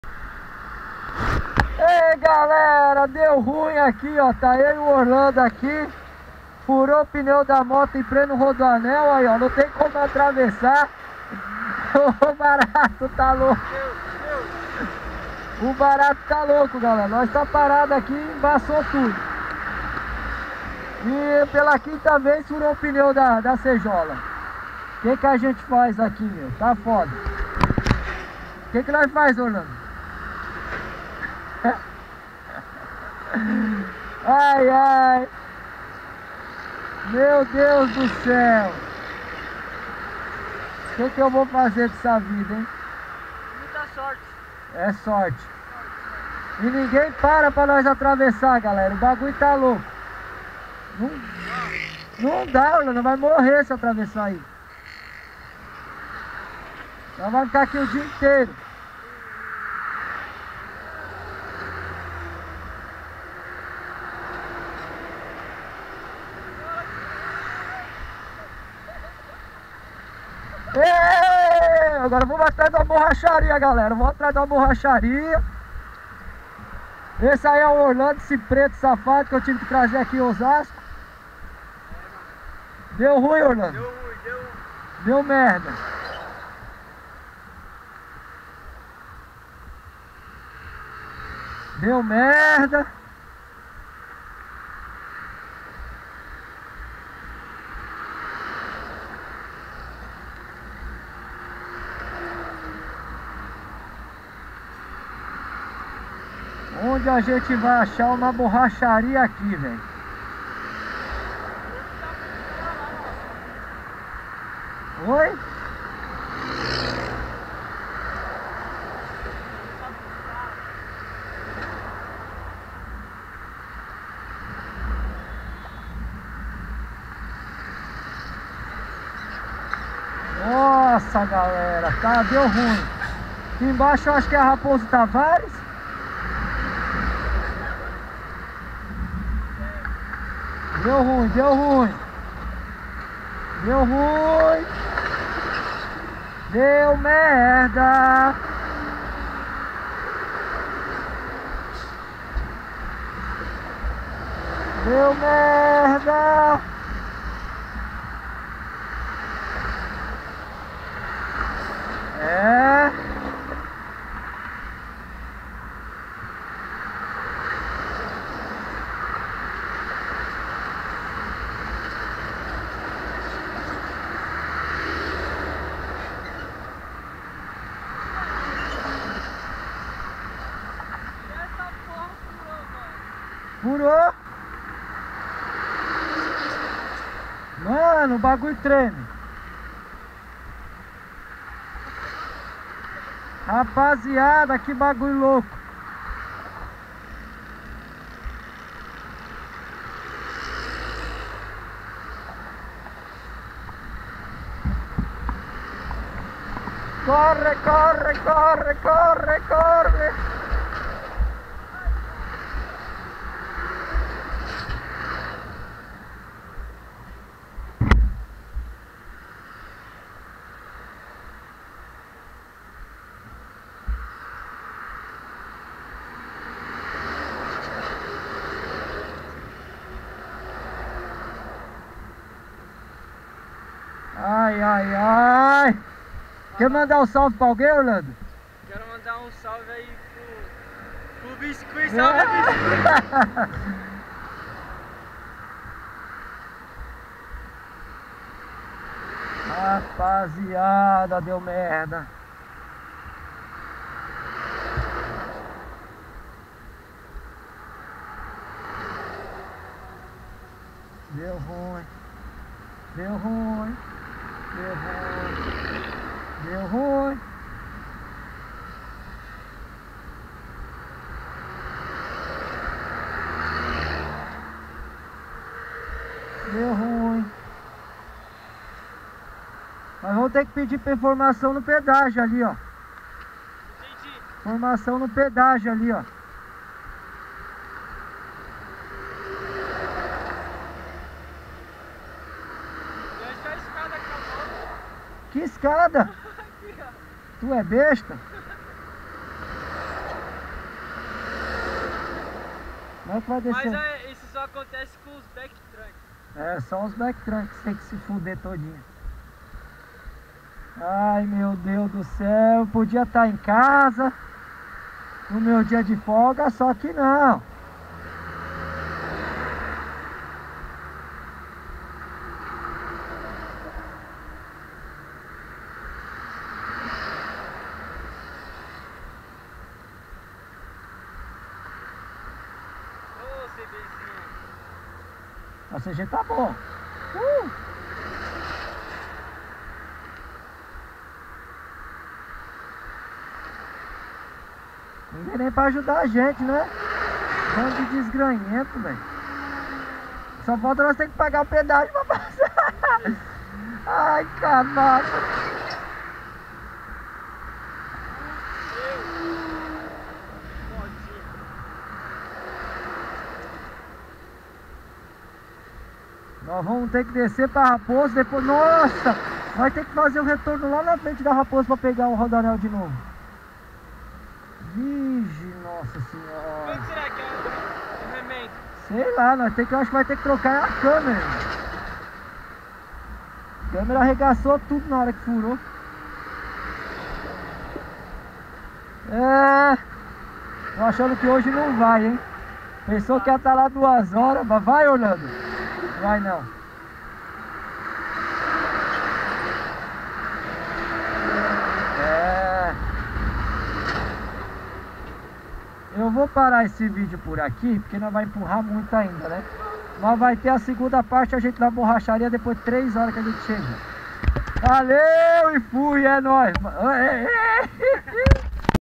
Pneu estourou no Rodoanel assistam a tarde 2
Quase caimos no Rodoanel  quem gostou desse vídeo curti e compartilha Tmj galera